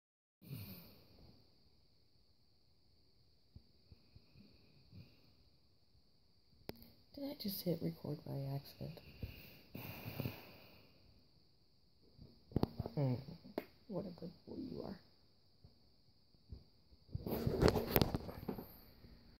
Accidental video of cuddling with Mathis in the morning
Mathis's 12 hours with me - 5th video